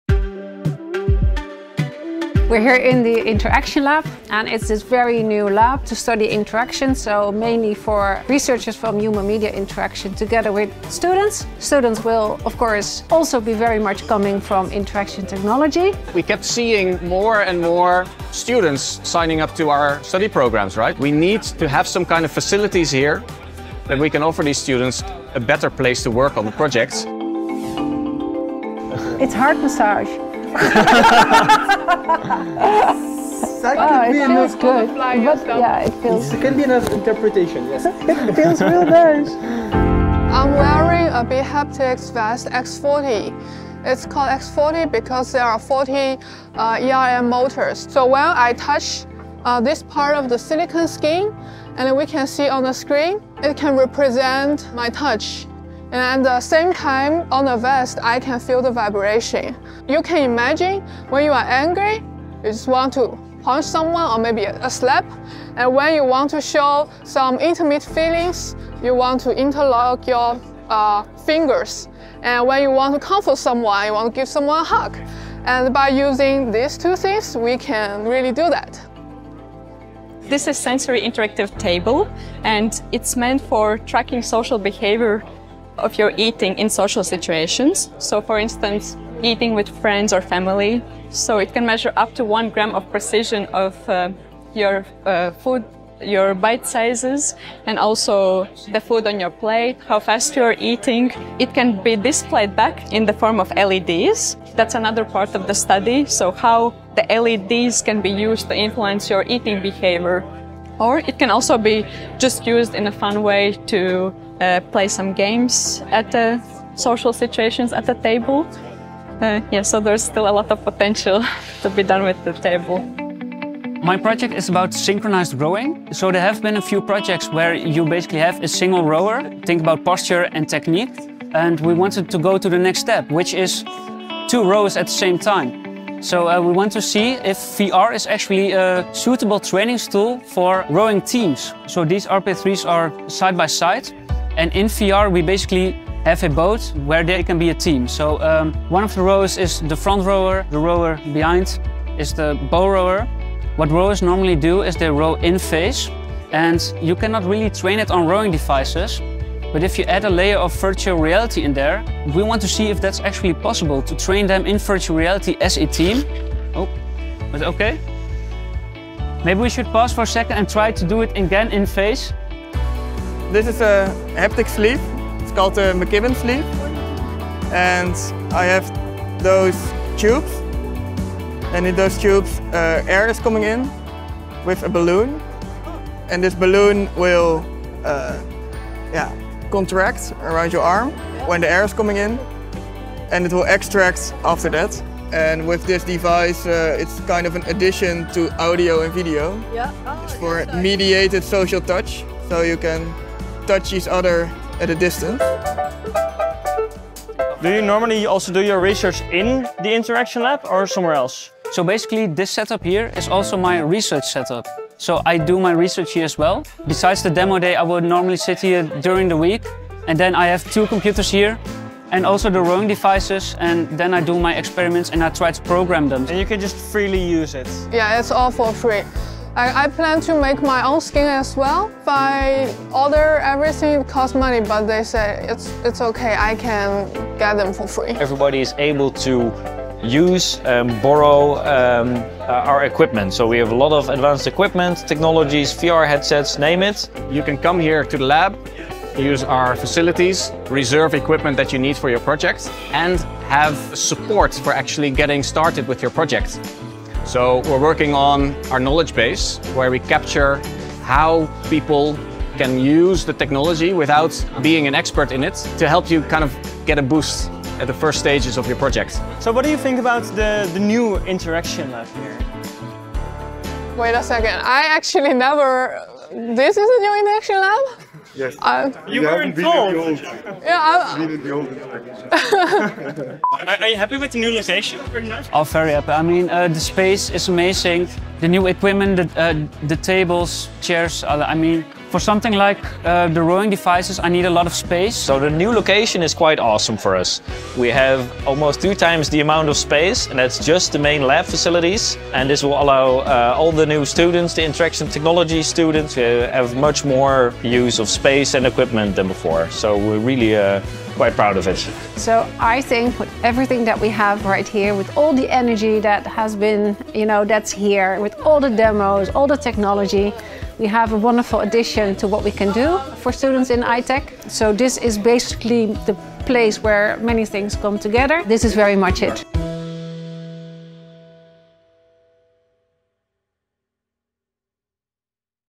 Interaction Lab facility for I-Tech projects
The Interaction Lab has moved to a new location: the Citadel and the experience so far is wonderful. Take a look at what facilities the lab offers. Rowing devices and VR research, the bHaptics vest X40, a haptic sleeve. The Interactionlab brings all oppertunities to do research for Human Media Interaction. With training sessions, projects, and a catalogue full of equipment you can borrow.

If you want to know more about it, you can look

Video produced by Seddle Productions for the University of Twente.